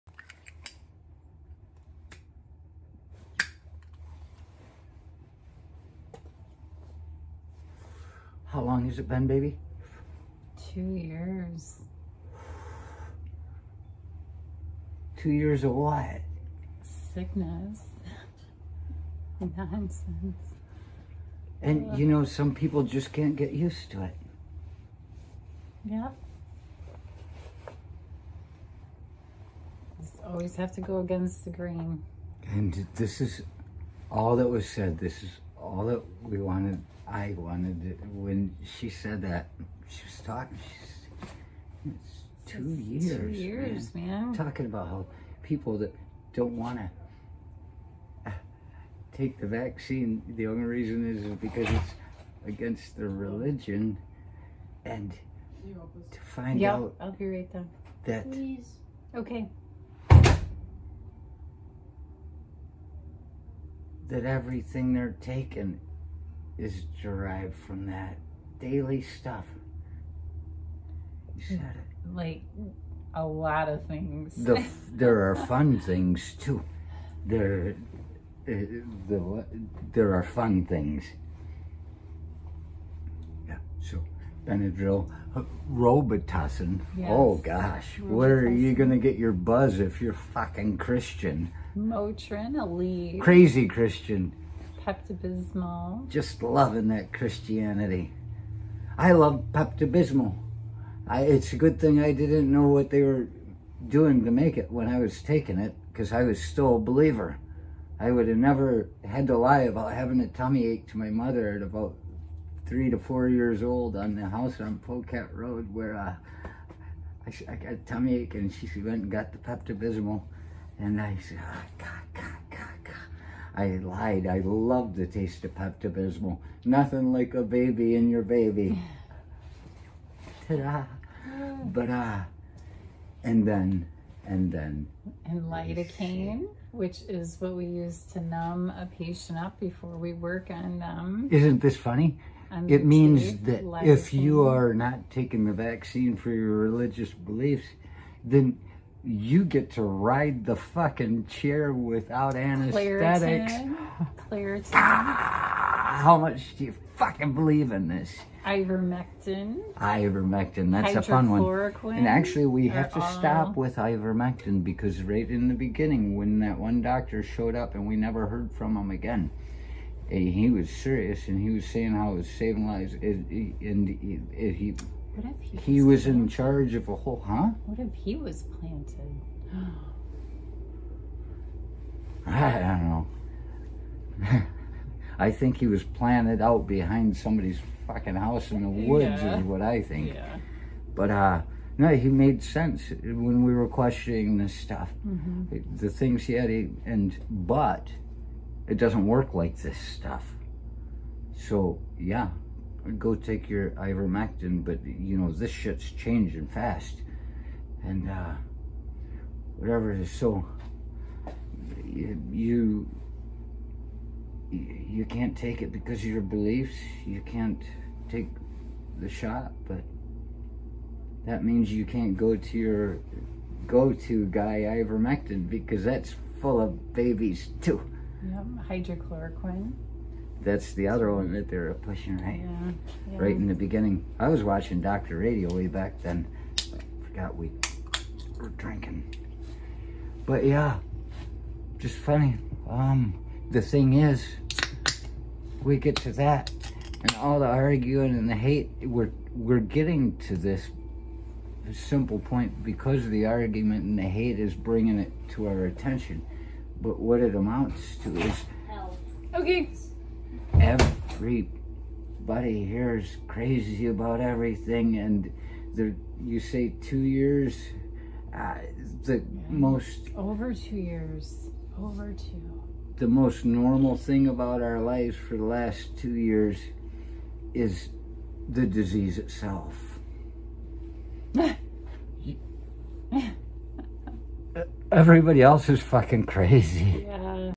“The Only Thing that is Normal” The Judas Resurrection
Metal dj speaks about such a thing as two years of normalcy